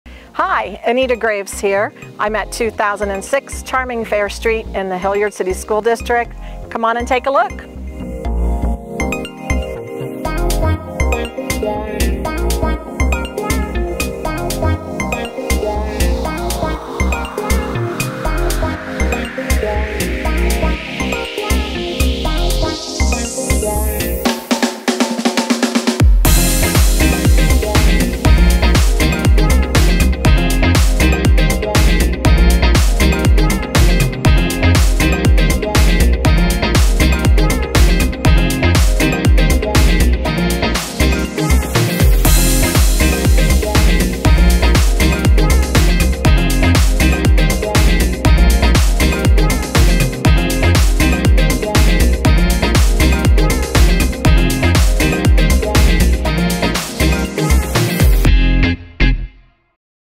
Home For Sale - 2006 Charmingfare St, Columbus, Ohio 43228
The Anita Graves Group and Corcoran Global Living present this new home for sale in the Hilliard, Ohio School District and Golfview Woods Neighborhood!

4 Beds | 3 Full Baths | 2,006 SQFT | 2 Car Garage

Welcome home to this unique floor plan in Hilliard Schools! This home offers 4 bedrooms and 3 full baths located in the highly desirable Golfview Woods community! New carpet in 2022, storage shed, new doors/trim upstairs and radon mitigation. Large fenced-in backyard to enjoy beautiful evenings. Hilliard Schools and Columbus taxes. Located close to Golf, shopping, grocery and highway access. This move-in ready home is perfect for entertaining with family and friends. A must see home in Columbus!